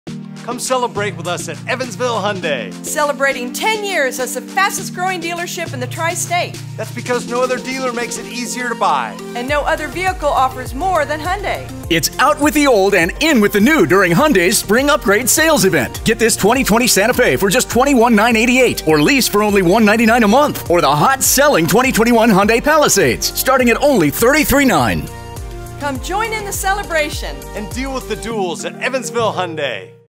Spring Upgrade Sales Event | Hyundai Santa Fe and Palisade | Evansville Hyundai
Come celebrate with us at Evansville Hyundai! Celebrating 10 Years as the fastest growing dealership in the tri-state.

That's because no other dealer makes it easier to buy. And no other vehicle offers more than Hyundai!  It’s out with the old and in with the new during Hyundai’s Spring Upgrade Sales Event.

Get this 2020 Santa Fe for just $21,988, or lease for only $199/month. Or the hot-selling 2021 Hyundai Palisades starting at only $33,900.

Come join in the celebration and Deal with The Duell's at Evansville Hyundai.